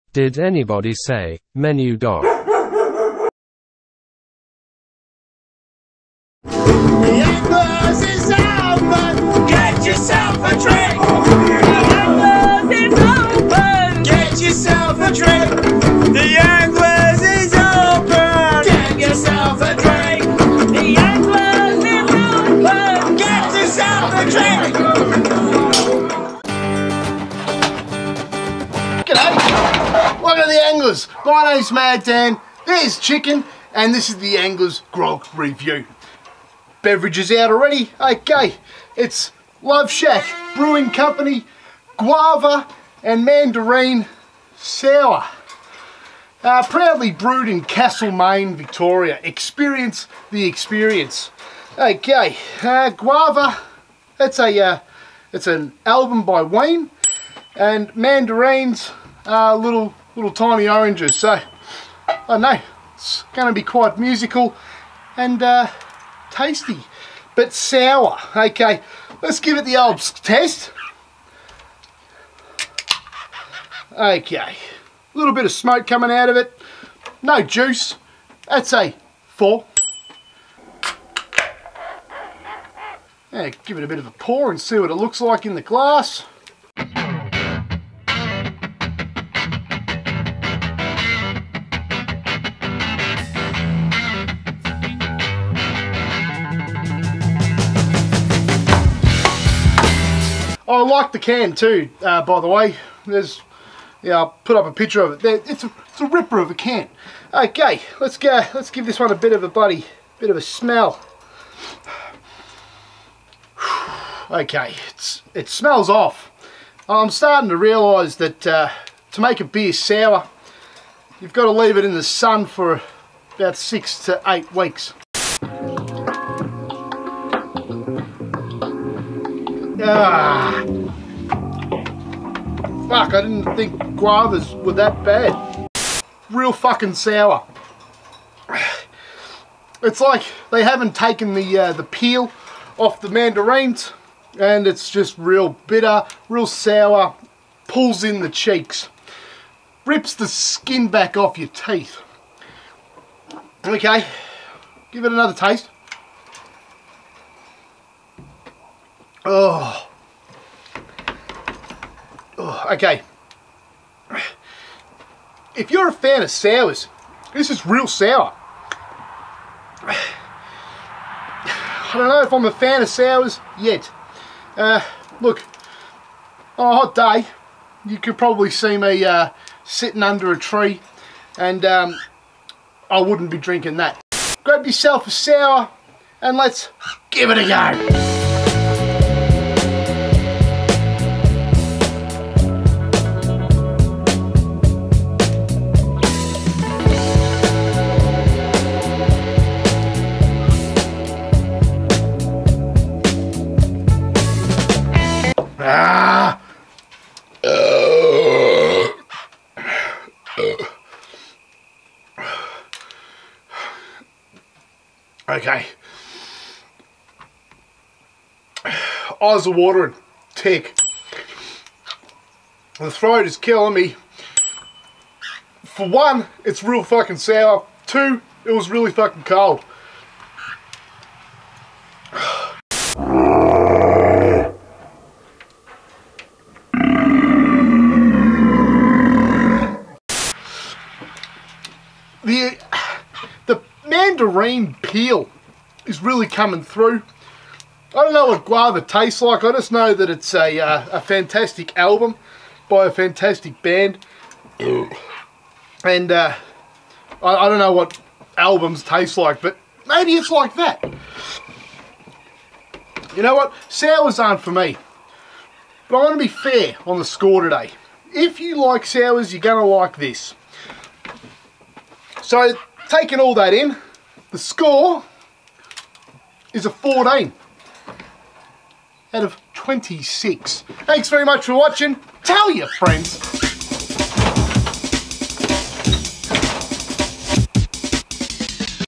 Love Shack | Guava and Mandarin Sour | the °anglers grog review | Beer Review
Mad Dan reviews Guava and Mandarin Sour by Love Shack Brewing Co.

*Opinions expressed are solely my own

Vocals by K Mad, Mad Dan, Dry Jam
Lyrics written by Mad Dan and Dry Jam